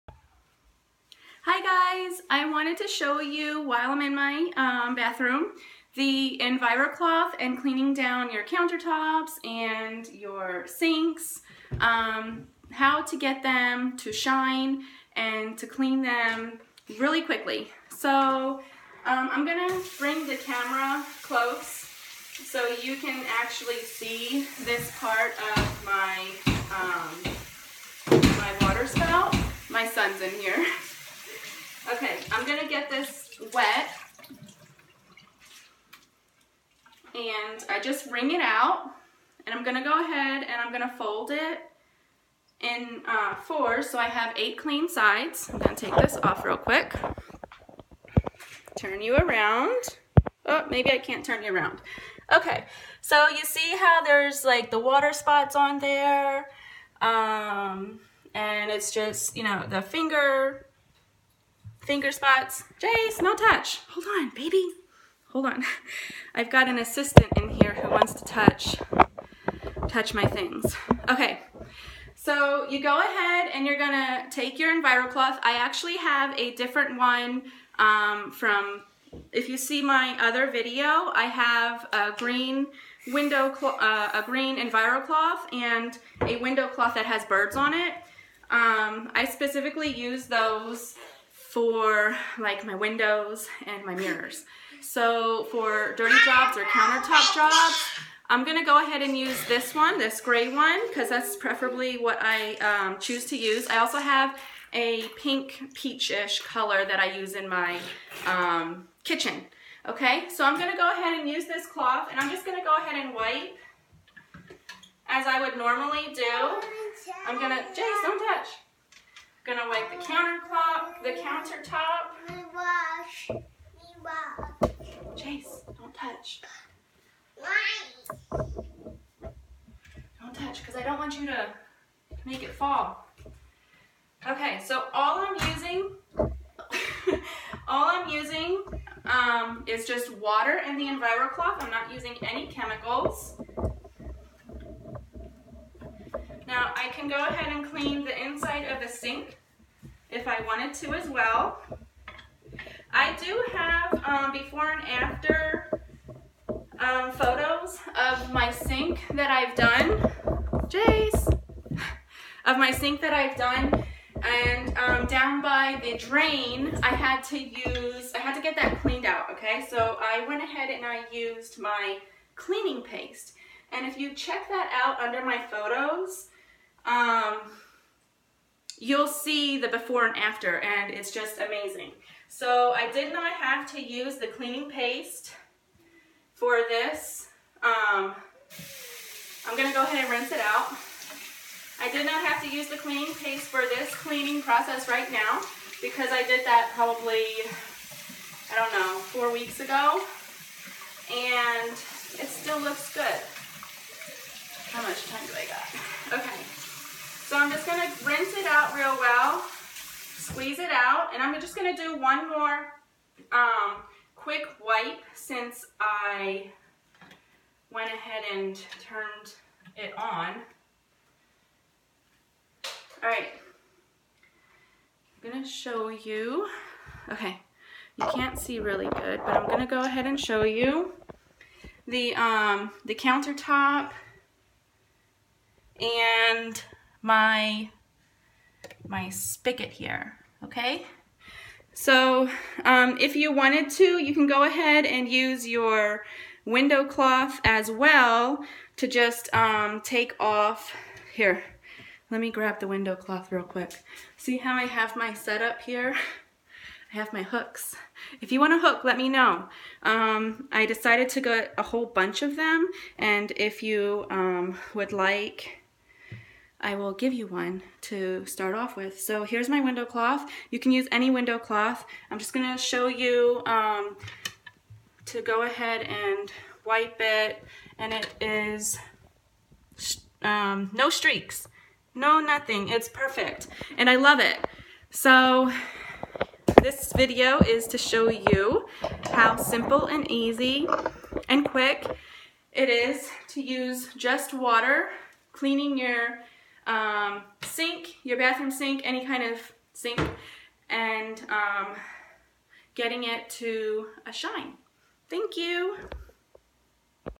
Let's Clean sinks with Microfiber and water
Ill show you how to clean your sinks, get those water marks off and have your hardware sparkling clean.  All with just water!